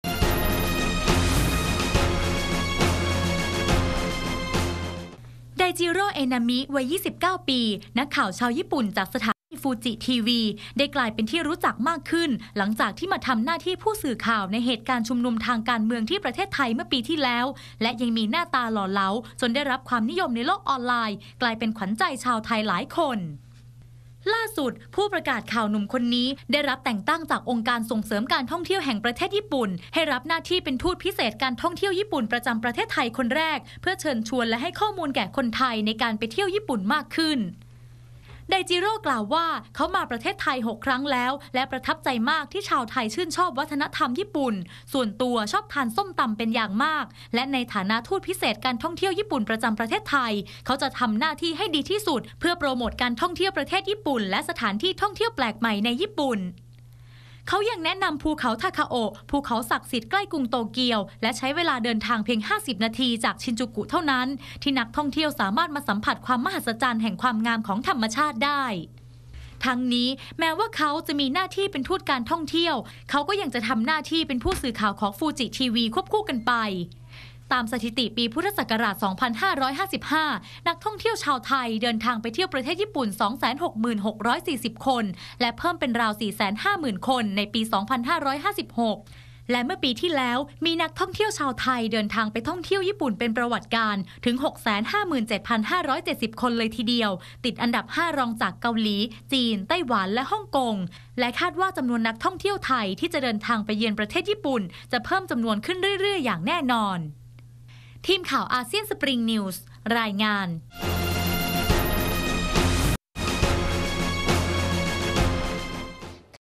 ญี่ปุ่นตั้งนักข่าวทีวีรูปหล่อเป็นฑูตท่องเที่ยว ดึงคนไทยเข้าประเทศ - Springnews
"ไดจิโร่ เอนามิ" นักข่าวหนุ่มญี่ปุ่นที่คนไทยคงรู้จักกันเป็นอย่างดี เจ้าของฉายา "หล่อทะลุม็อบ" จากเหตุการณ์ชุมนุมทางการเมืองในประเทศไทยเมื่อปีที่ผ่านมา ได้รับการแต่งตั้งให้เป็นทูตพิเศษการท่องเที่ยวญี่ปุ่นประจำประเทศไทยคนแรก รายละเอียดจะเป็นอย่างไร ติดตามได้จากรายงาน "อาเซียนอัพเดท"